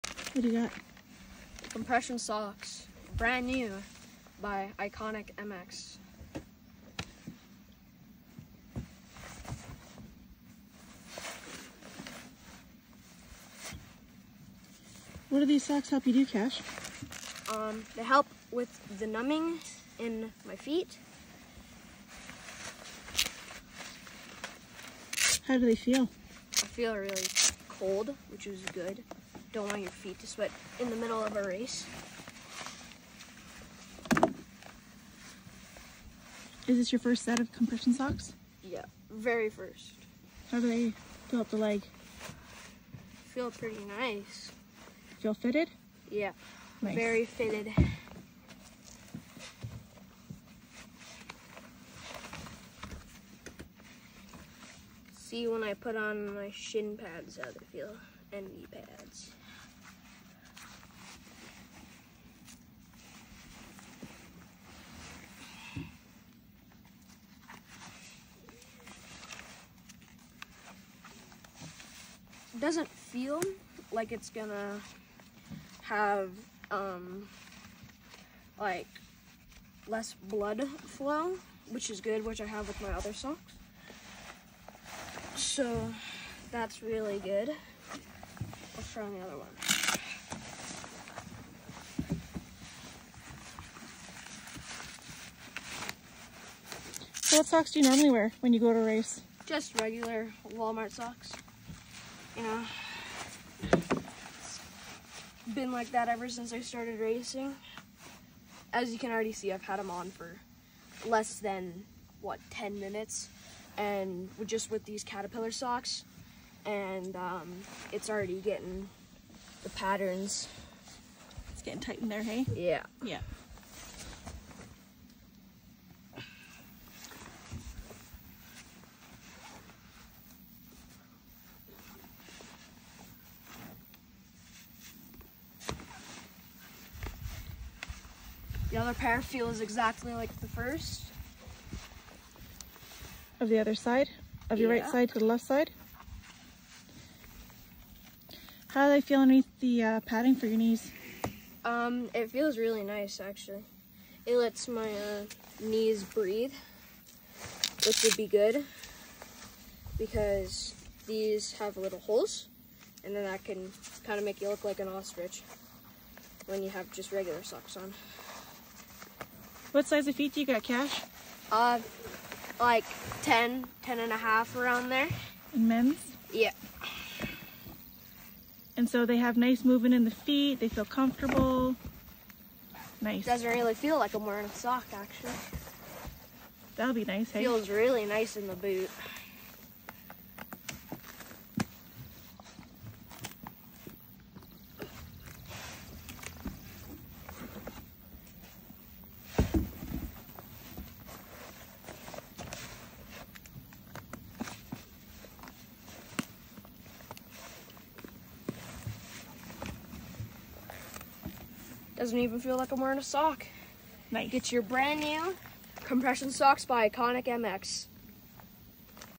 Iconic MX Athletic Compression Socks Review
Cash Clay #11, gives his honest review of Iconic MX’s newest product launch, 15-20MMGH level compression athletic socks.

Perfect during any athletic performance where circulation and pressure are necessary.